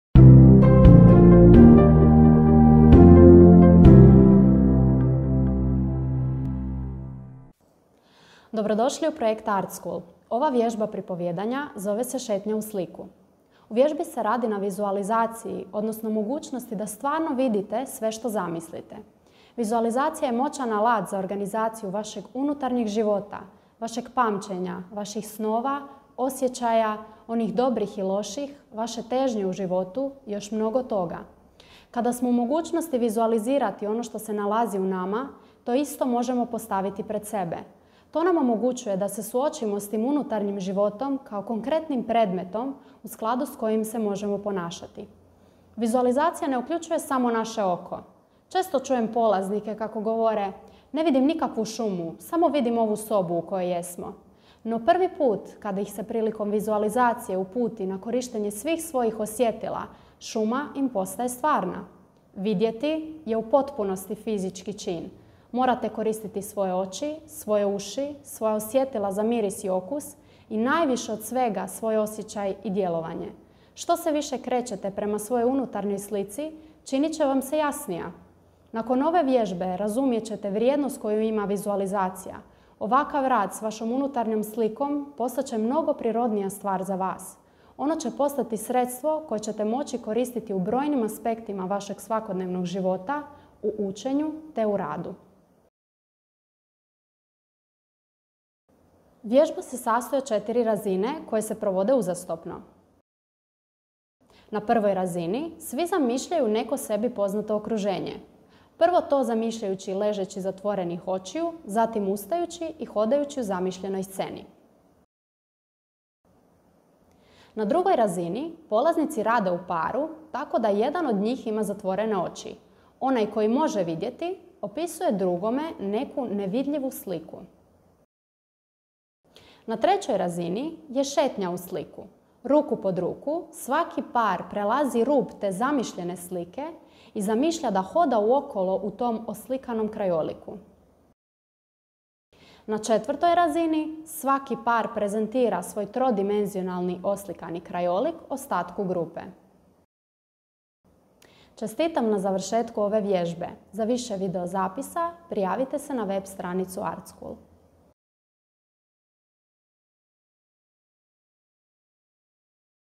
Artskul - IO1 - Intro speak - A Walk into the Painting - Storytelling - FO - HR
Dobro došli u projekt Artskul.

Ova vježba pripovijedanja zove se Šetnja u sliku.

Za više videozapisa posjetite mrežne stranice projekta Artskul - artskul.eu.